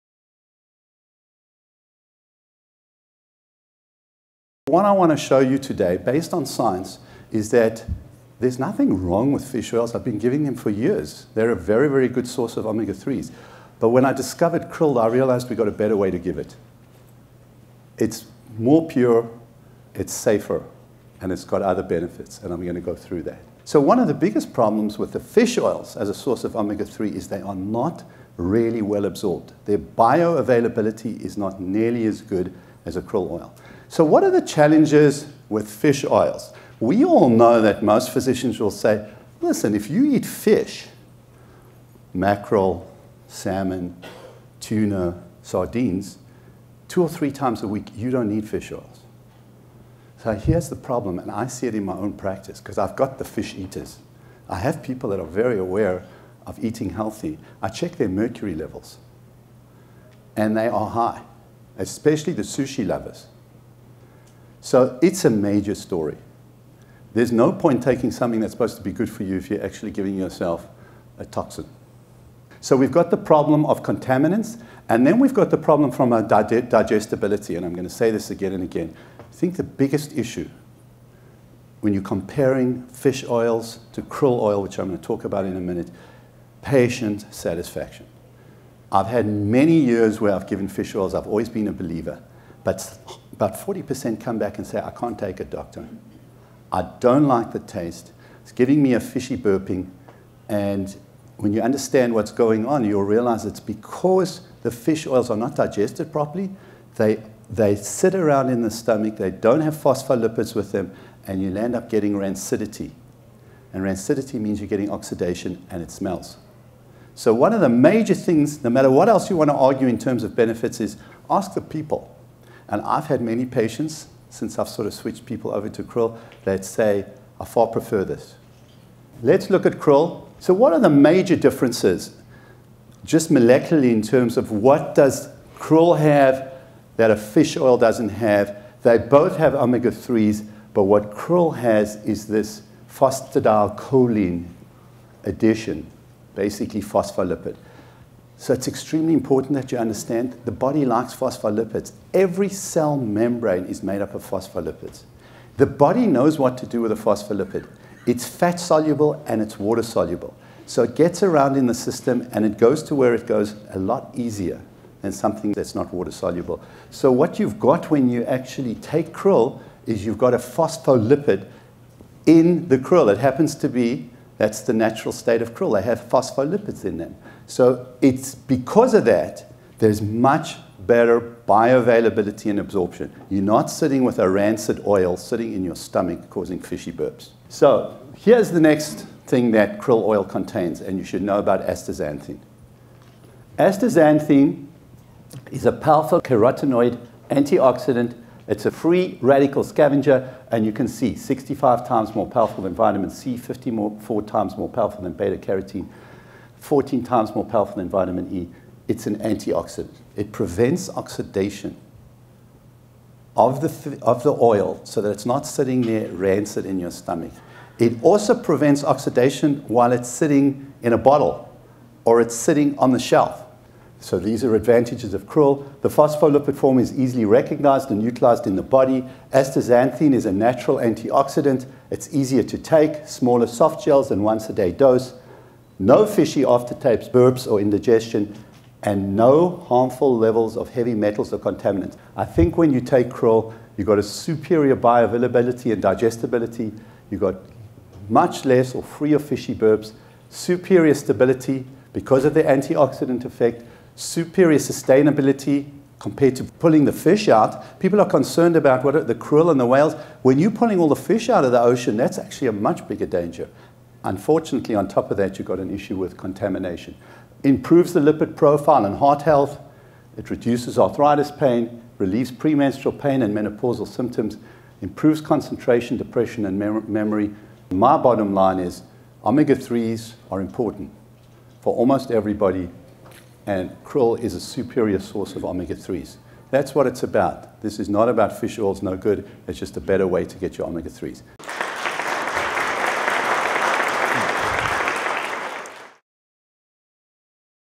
The Thrill of Krill - Dr. Dennis Goodman
Best-selling author and noted cardiologist Dr. Dennis Goodman provides a clear understanding of why krill oil is surpassing fish oil as a superior supplemental source of omega-3